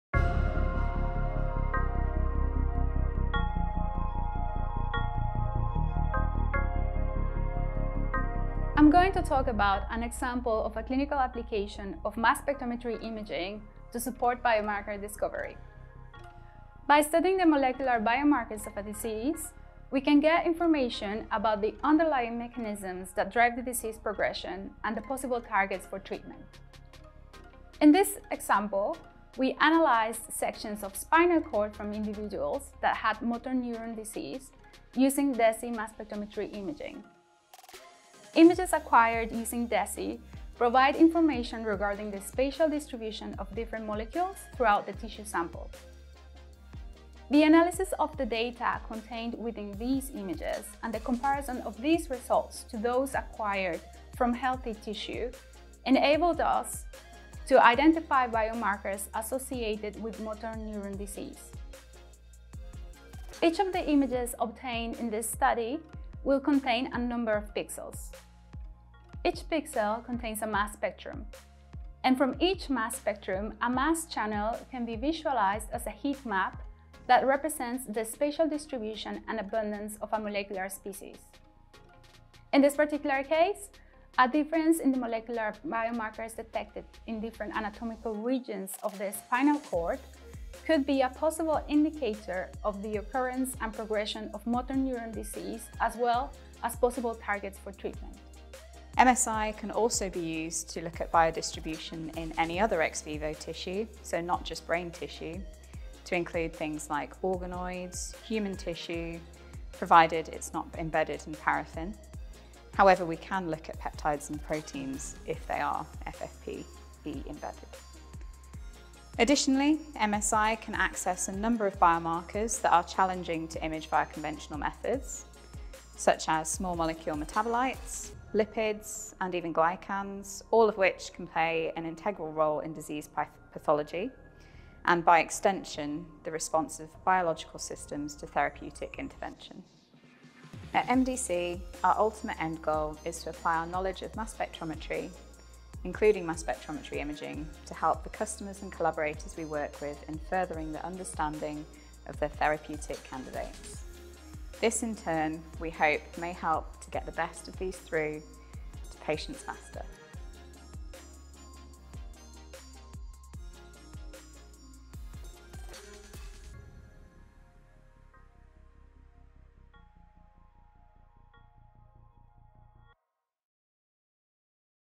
The Application of Mass Spectrometry Imaging at Medicines Discovery Catapult
An example of a clinical application of mass spectrometry imaging to support biomarker discovery by studying the molecular biomarkers of a disease.

We can get information about the underlying mechanisms that drive the disease progression and the possible targets for treatment.

In this example, we analysed spinal cord sections from individuals with motor neurone disease.